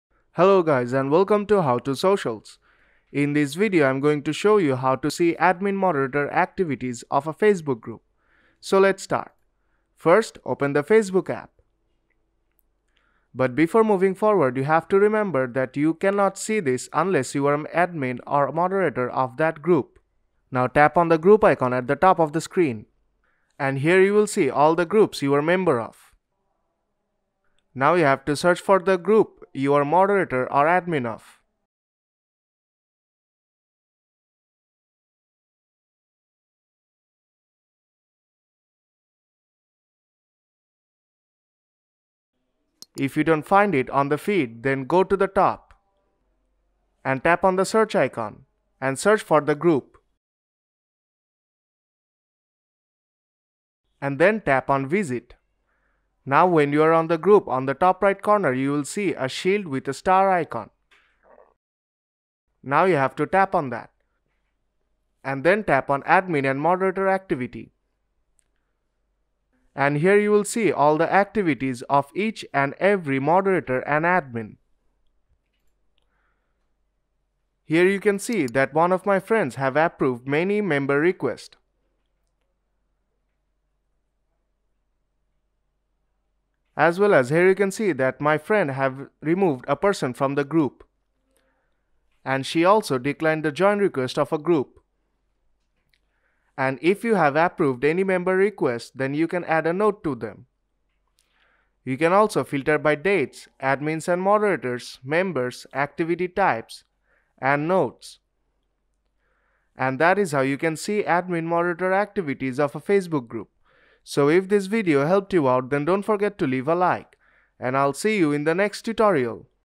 How to See Admin Moderator Activities of a Facebook Group (VERY QUICK)
How to See Admin Moderator Activities of a Facebook Group

In this video I show you how to see admin moderator activities of a facebook group in 2021.